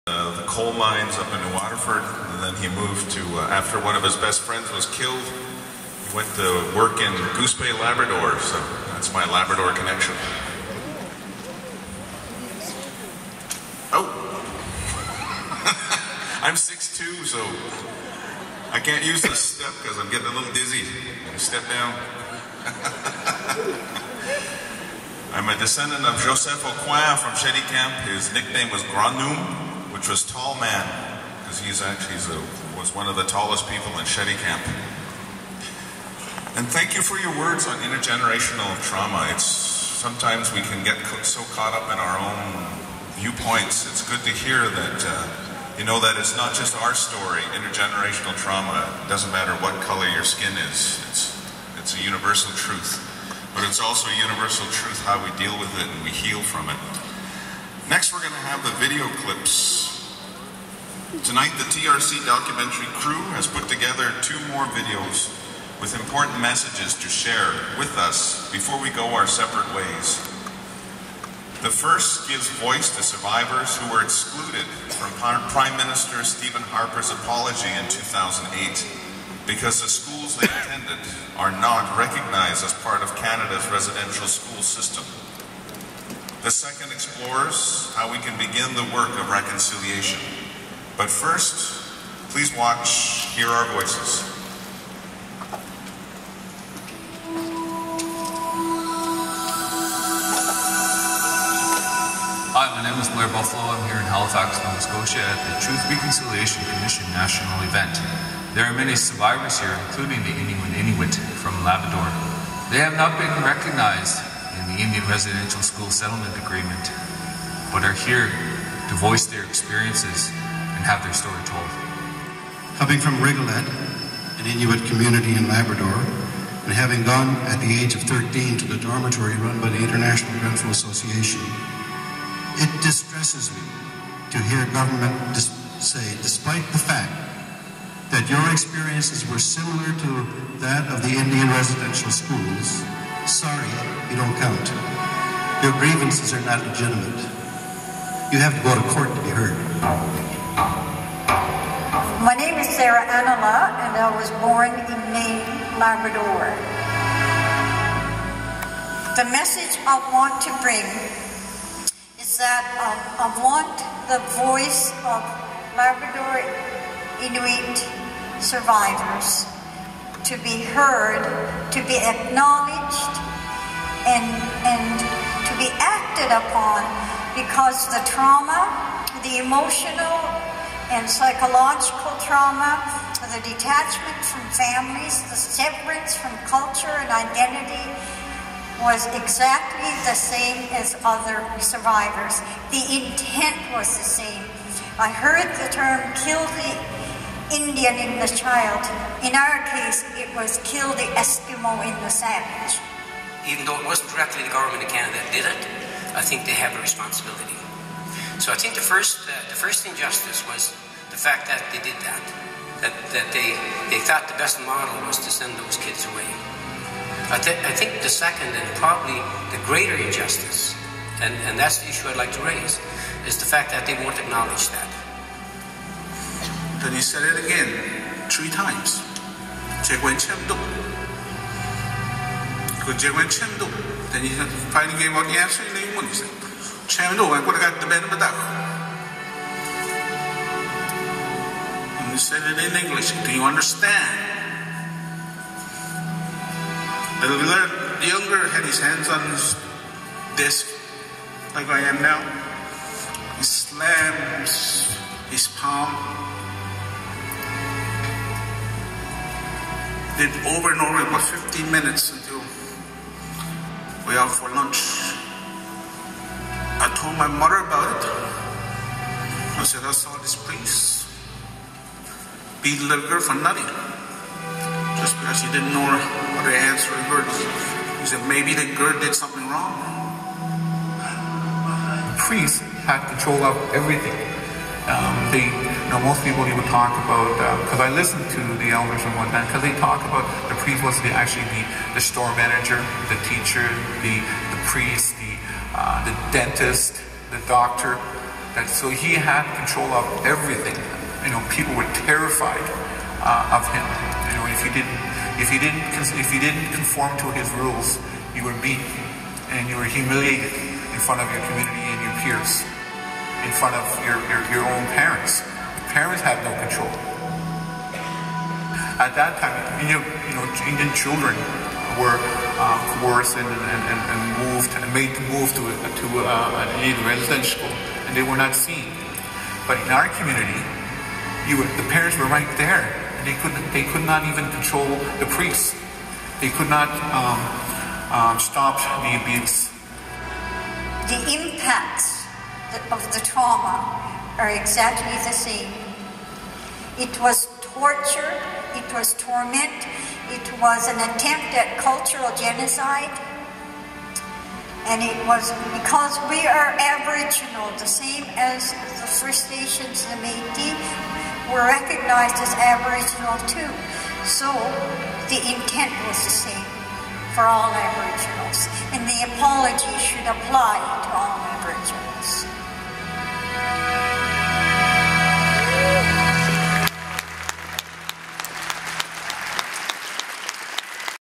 Native Truth & Reconciliation (03) Hear our voices
Link to full article, photos and videos.

This is  the last days Native Truth Reconciliation events in Halifax Nova Scotia Canada

This is about the horrific abuses suffered by the Canadian Native peoples across the country. READ THIS ARTICLE Residential School Survivors Share Their Stories I also lived at one of these residential schools. In my article I called it an orphanage. please read it here;
 I did not go to the testimonies as I am sure I would of cried through the whole thing.. I remember very clearly everything I suffered at the hands of the Catholic nuns.. I recommend you watch
Be sure to watch my International POW WOW series of videos